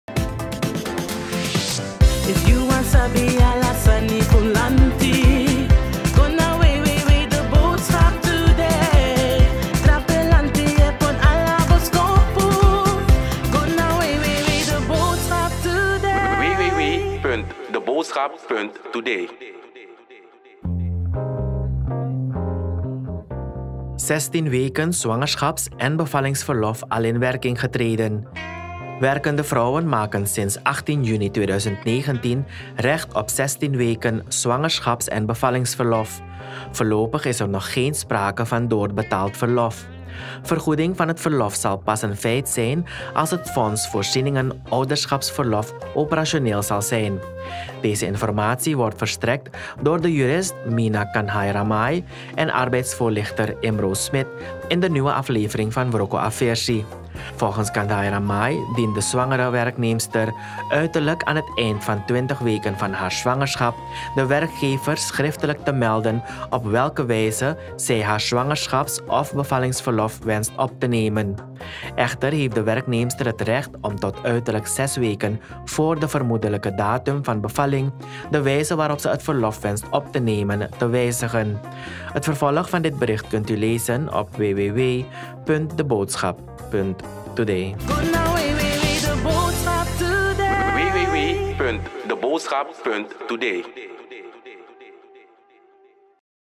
16 weken zwangerschaps en bevallingsverlof al in werking getreden
Hier de link van het bericht: "16 weken zwangerschaps en bevallingsverlof al in werking getreden"